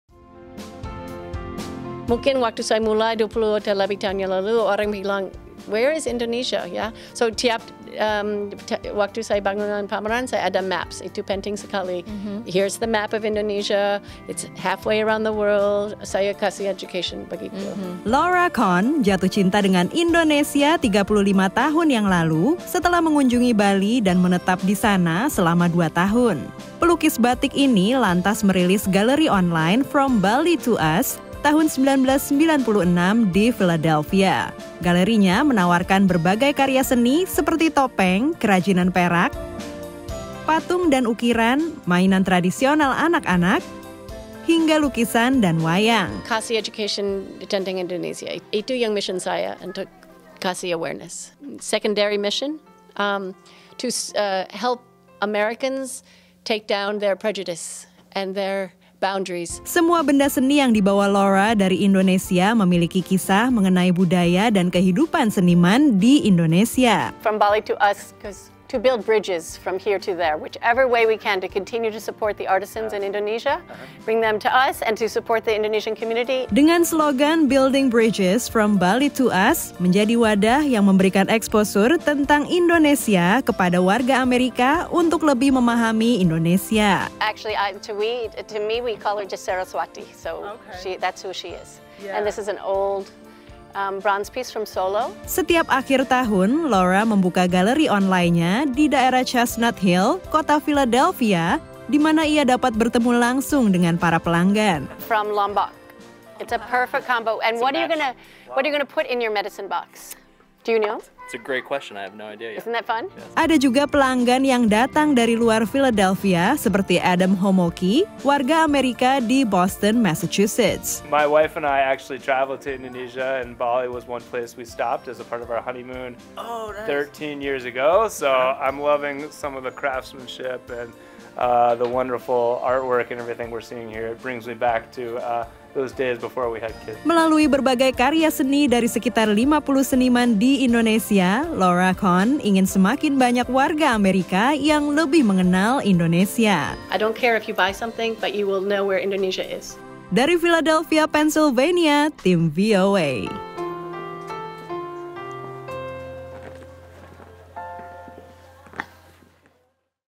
Menjembatani Amerika dan Indonesia, Laura Cohn Perkenalkan Indonesia Melalui Seni
Dengan misi menjembatani Indonesia dan Amerika, Laura Cohn, Warga AS di Phildelphia membangun bisnis “From Bali To Us”. Toko online yang dirintis sejak 1996 ini, menawarkan berbagai karya seni dari Indonesia dengan kisah budaya dan kehidupan seniman pembuatnya.